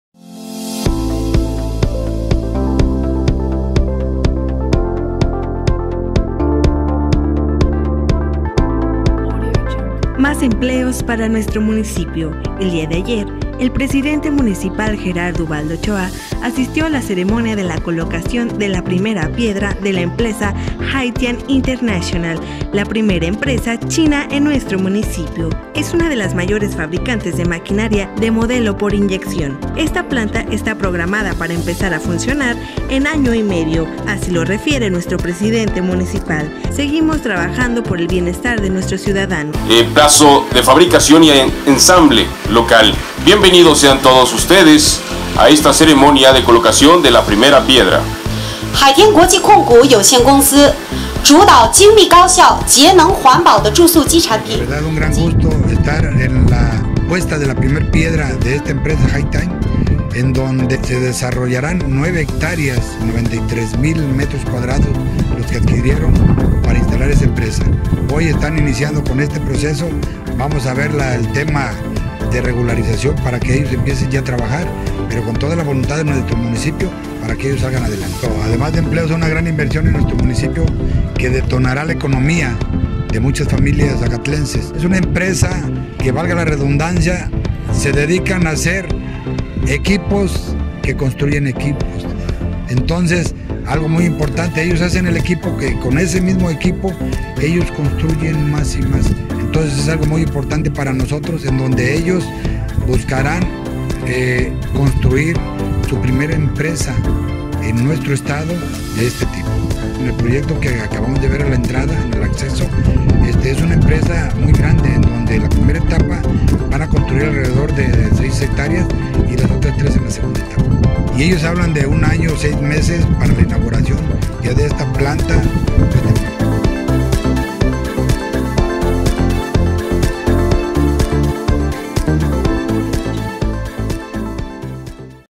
PRIMER PIEDRA, HAITIAN INTERNATIONAL 2019.
Más empleos para nuestro Municipio.
Haitian International Holdings Ltd, uno de los mayores fabricantes de maquinaria de modelo por inyección a nivel mundial, anuncia la construcción de uno de sus sitios de manufactura en América, como parte de su estrategia global de largo plazo que busca fabricar y ensamblar equipos de maquinaria local.
El día de ayer el Presidente Municipal Gerardo Uvaldo Ochoa y parte de su equipo de trabajo, asistieron al Centro Logístico Jalisco, en donde se llevó a cabo la ceremonia de la colocación de la primera piedra.
Esta planta está programada para empezar a funcionar, aproximadamente, en un año y medio.
Seguimos trabajando por el bienestar de nuestro Municipio.